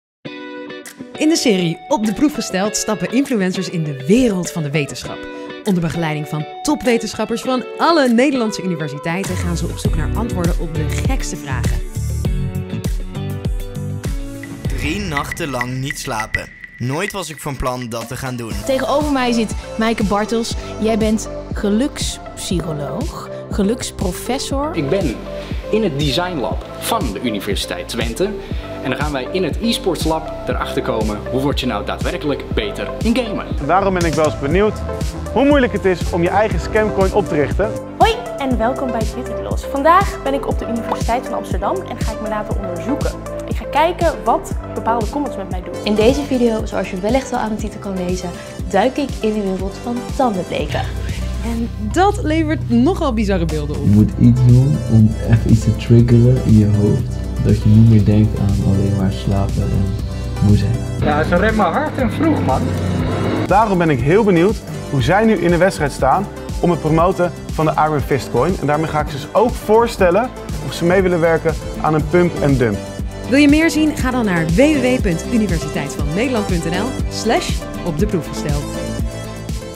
Op de Proef Gesteld: Youtubers duiken in de wereld van de wetenschap
In onze nieuwe serie Op de Proef Gesteld stappen influencers direct in de wereld van de wetenschap. Onder begeleiding van topwetenschappers van de Nederlandse universiteiten worden ze op de proef gesteld door de wetenschap in hun zoektocht naar antwoorden op de bizarste vraagstukken.